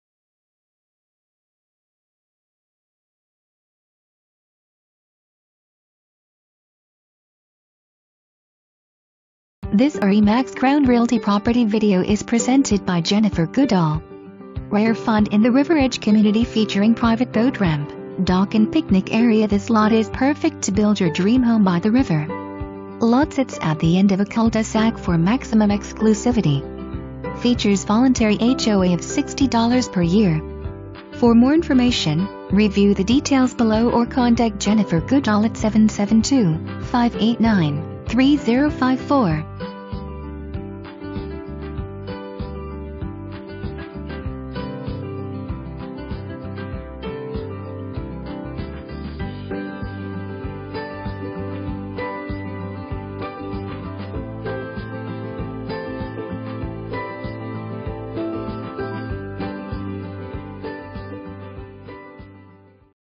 RE/MAX Crown Realty - 34 Sunset Drive
Please enjoy this property video created by RE/MAX Crown Realty

Rare find in the River Edge Community featuring private boat ramp, dock & picnic area this lot is perfect to build your dream home by the river! Lot sits at the end of a cul de sac for maximum exclusivity! Features voluntary HOA of $60 per year.

Jennifer Goodall
RE/MAX Crown Realty